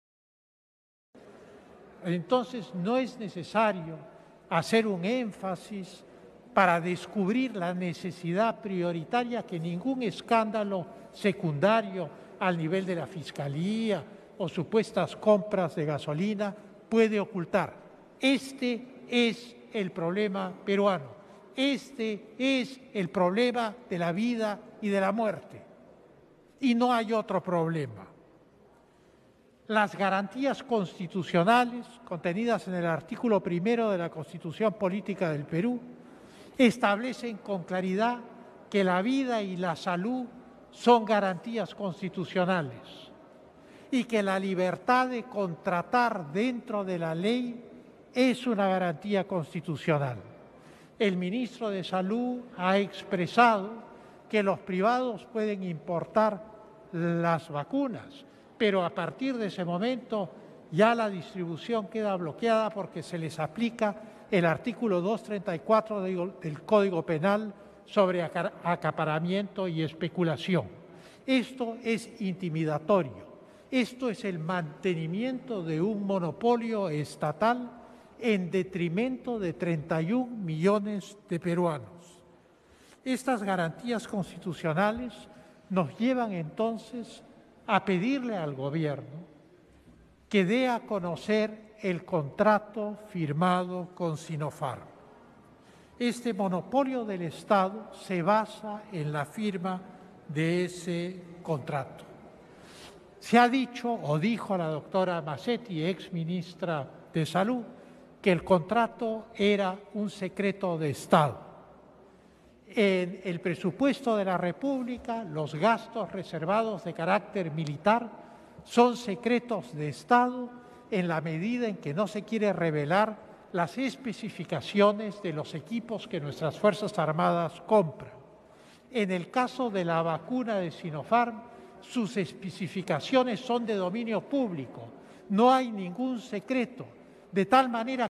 Francisco Tudela exige a Sagasti deja importar vacunas
Francisco Tudela, Canciller del Gabinete de Oposición de Hernando de Soto, exige al gobierno morado de Sagasti respetar la Garantía Constitucional del sector privado para contratar, importar y distribuir las vacunas, y pide dar a  conocer el Contrato con Synopharm.